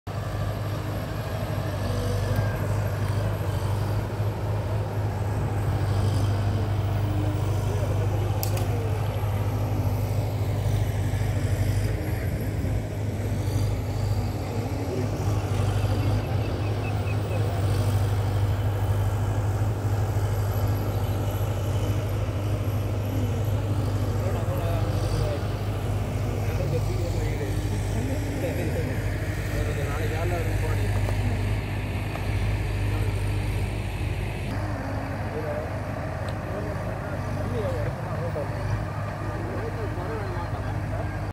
16 July 2021
excavation and rehabilitation uppu kulam in ayakkaranpulam muthaliyar kuthagai